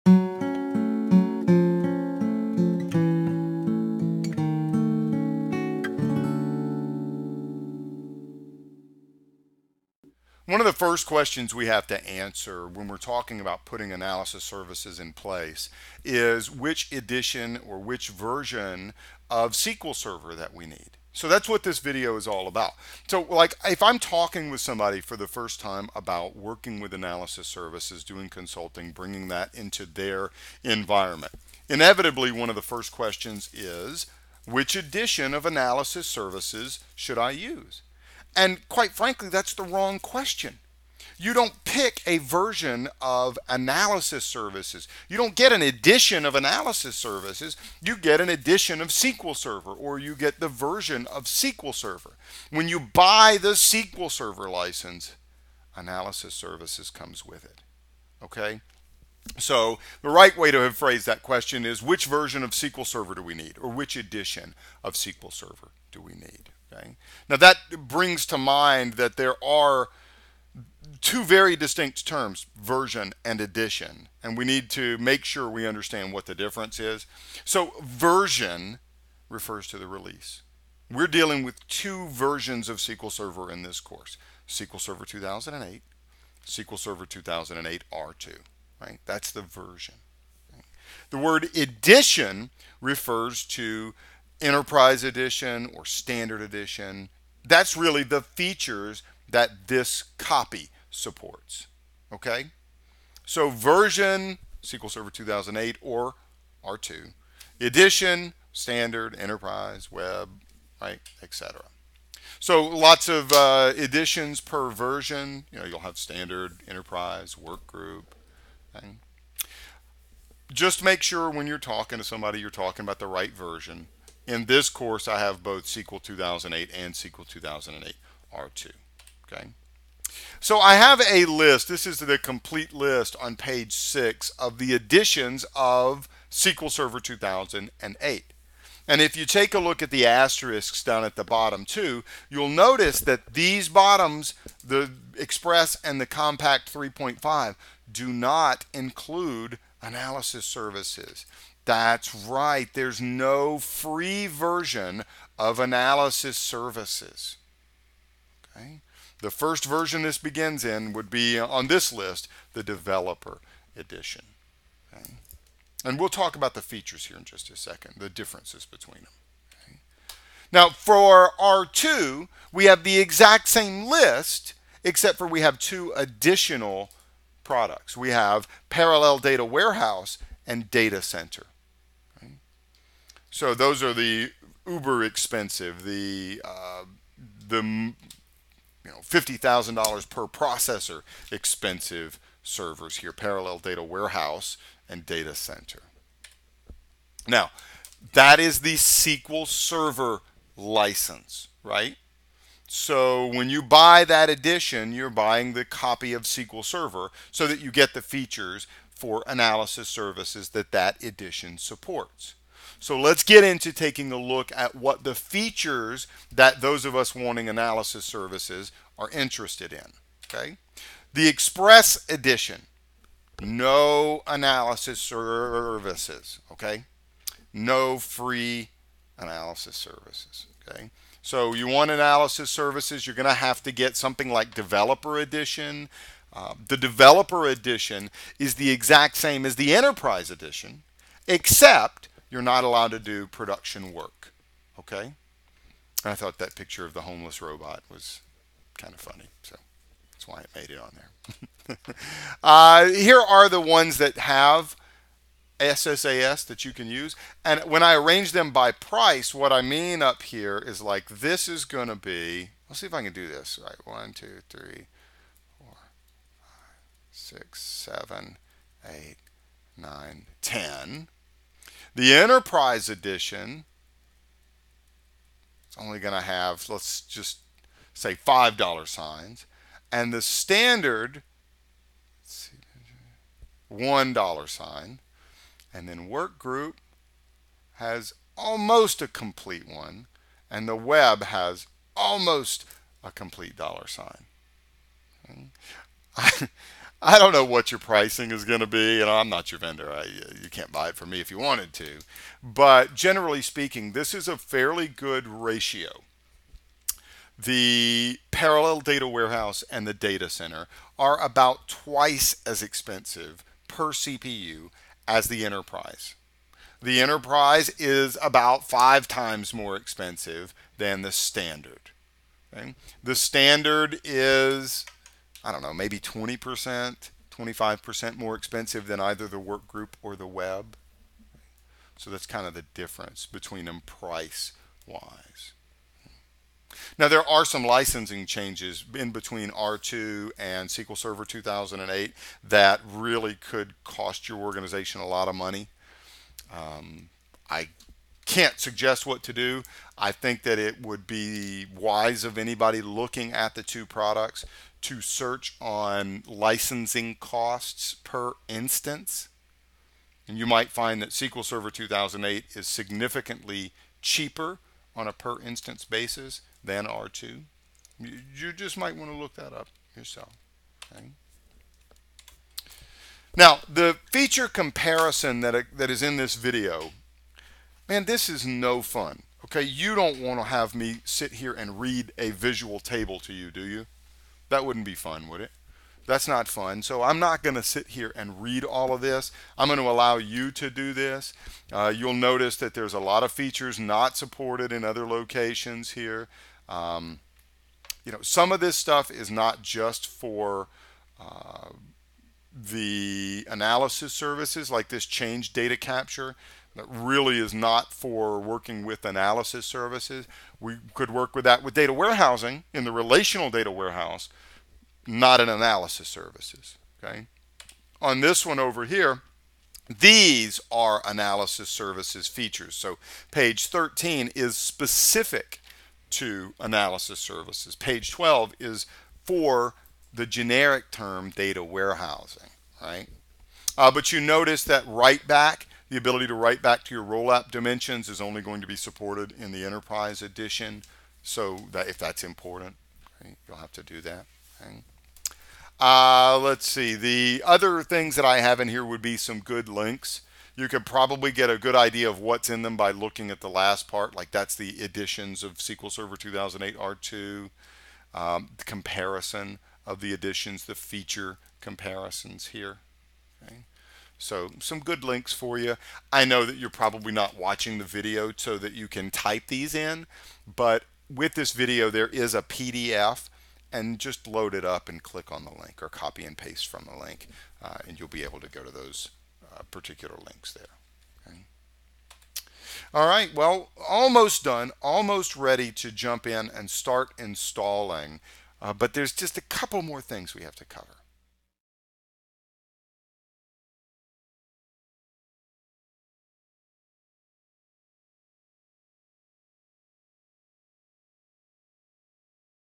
SSAS Basics: Editions of SQL Server 2008 and SQL Server 2008 R2
This video is part of LearnItFirst's SQL Server 2008/R2 Analysis Services course. More information on this video and course is available here:

In this video Scott will go over the different editions and versions of SQL Server 2008 and SQL Server 2008 R2.  This video will help you decide which version or edition of SQL Server you need.

Highlights from this video:

- Version vs. edition - what's the difference?
- A version will have many editions
- SQL Server Express Edition
- Developer Edition
- Comparing editions